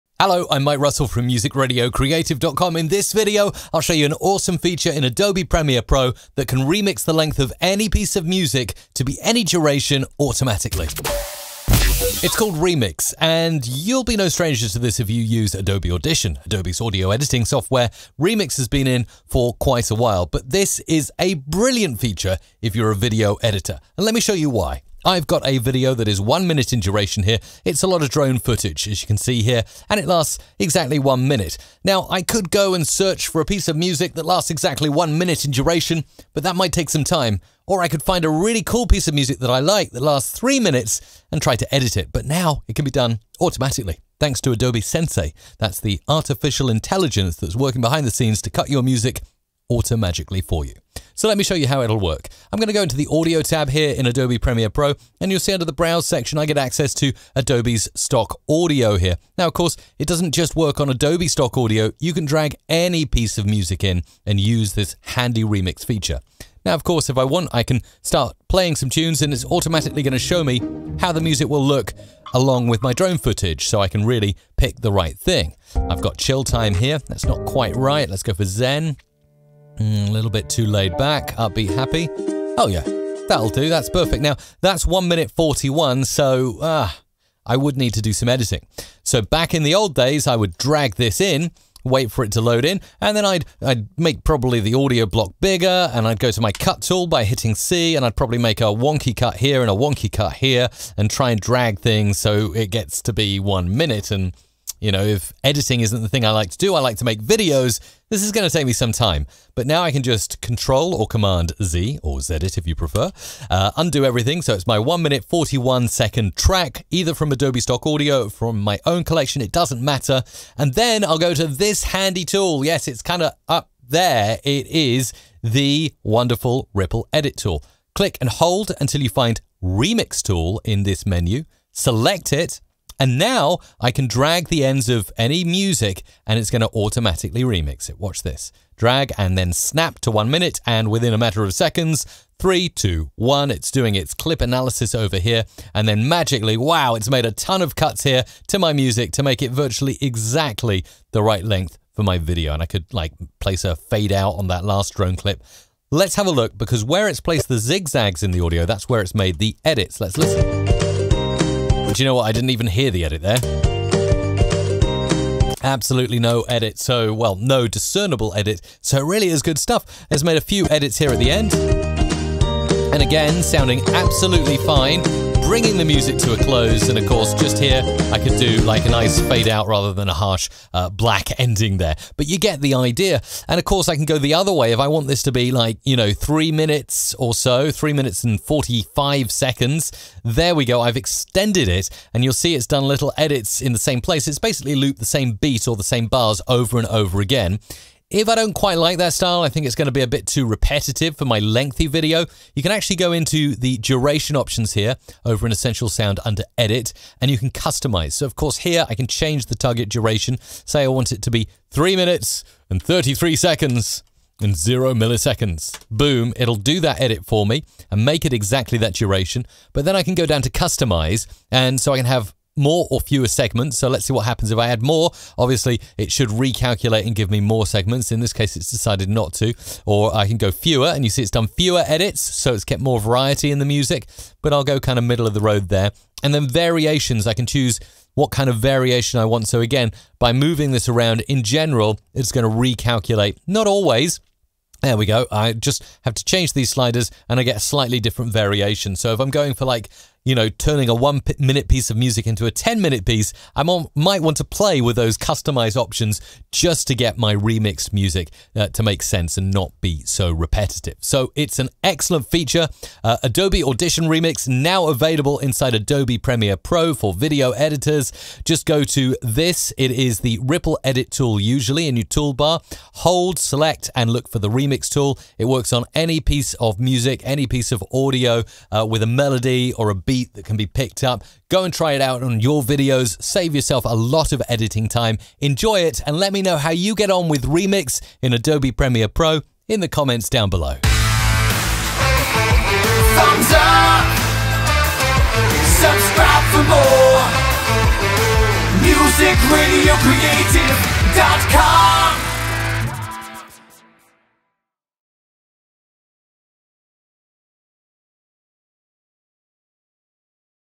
Adobe Premiere Pro CC - Remix Feature - Make Music ANY Length In SECONDS!
Cut music to ANY LENGTH in seconds inside Adobe Premiere Pro CC. No more harsh endings, no more nasty fades - THE AWESOME Remix feature has travelled from Adobe Audition and is now available inside Premiere Pro. Save yourself a ton of time and start making amazing music edits right inside the video editor.

◀ 🎙 Adobe Premiere Pro Presets! 🎧 The audio presets I use.

0:01 Tutorial Start
0:13 Remix feature introduction
0:26 Explaining the issue
0:50 Adobe stock audio selection (or alternative)
1:24 Finding the right music. Process in the past
1:49 Enable remix inside Adobe Premiere Pro
2:14 Drag ends of any music track to remix automatically
2:39 Checking the edits on the music tracks
2:59 Inserting soft fades
3:03 Extending the track with remix feature
3:40 Tweaking specific settings of remix feature
4:22 Tutorial wrap up